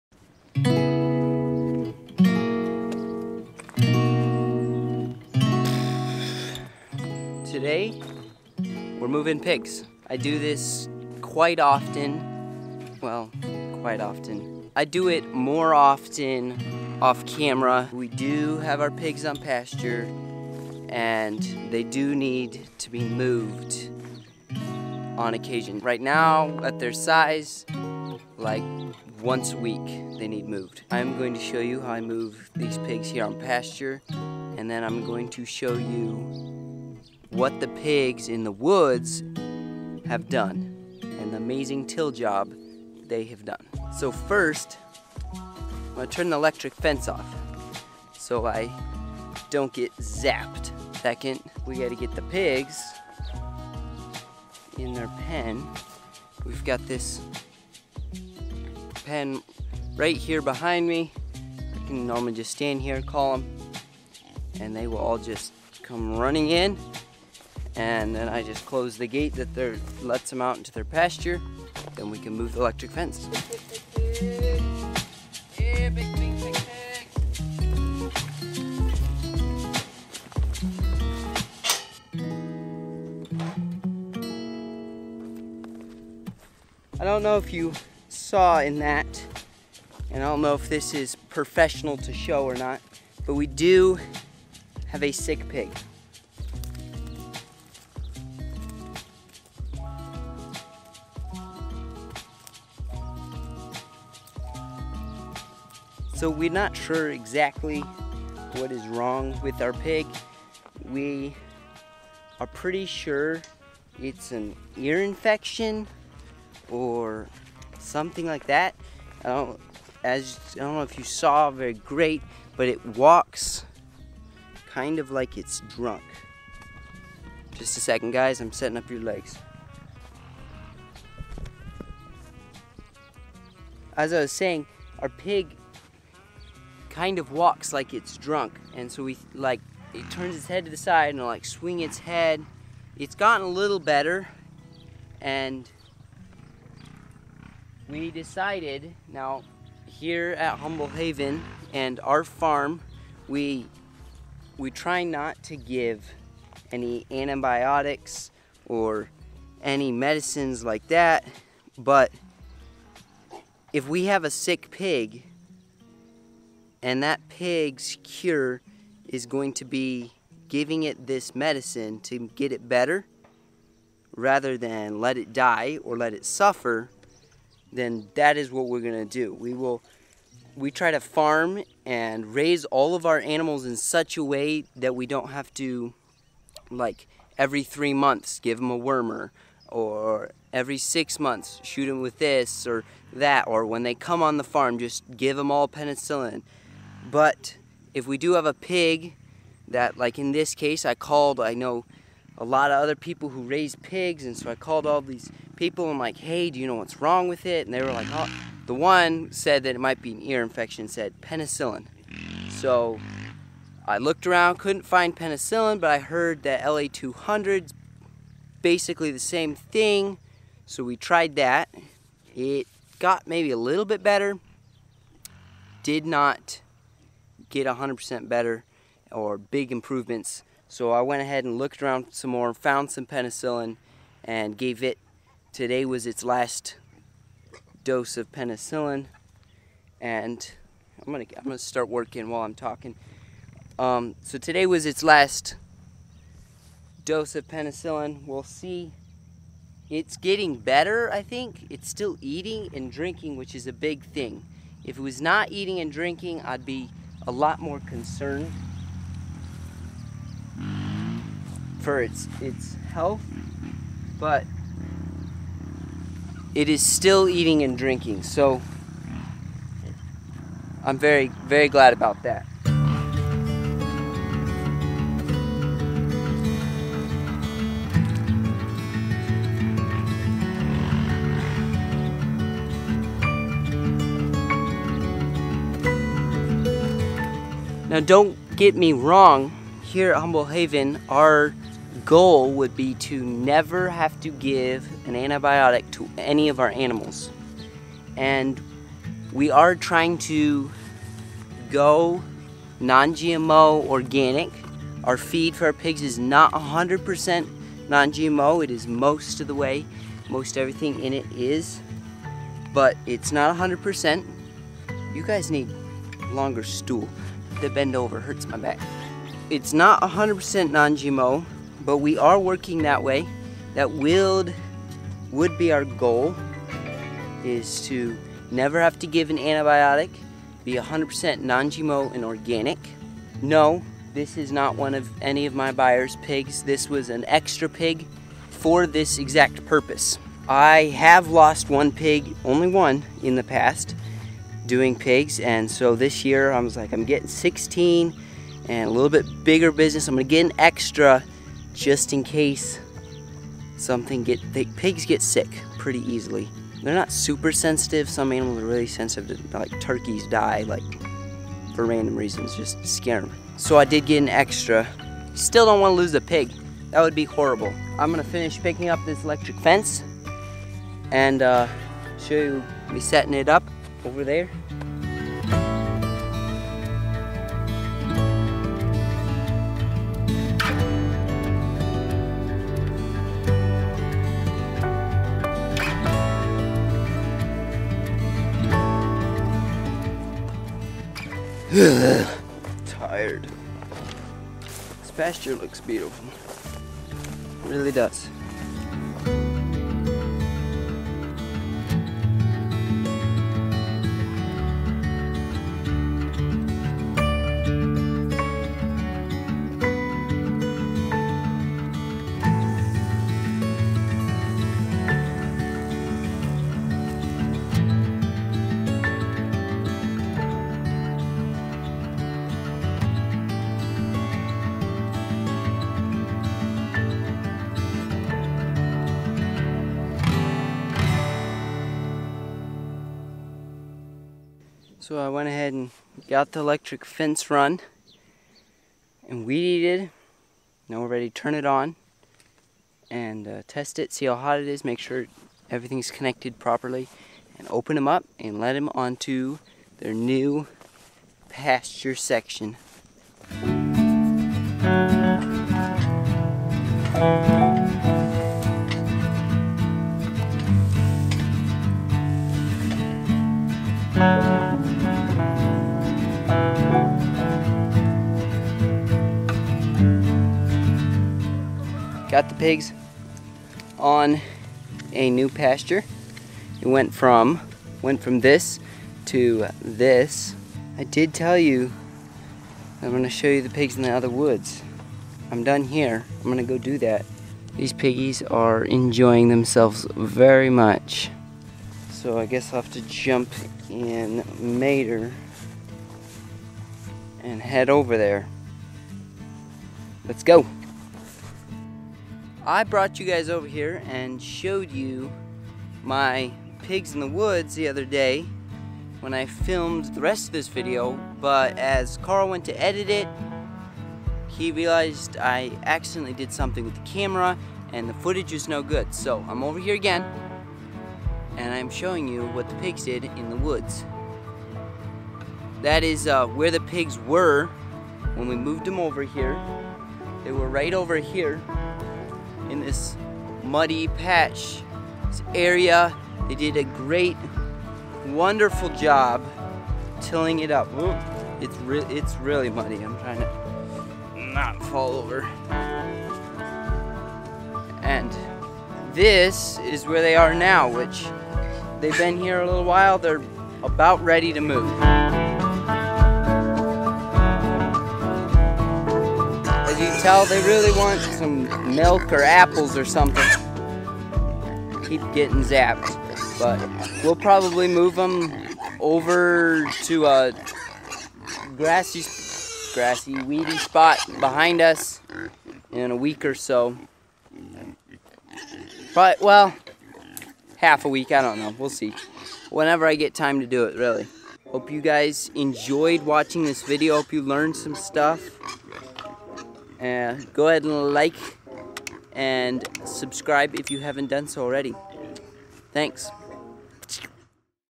Our Rotating Pasture System for Pigs.
Rotating pastures prevents overgrazing. Pastured pork is one of many aspects of what we do at Humble Humble Haven Farm.
A huge thank you to Sean Cannell and the entire Video Ranking Academy team for all their support and encouragement.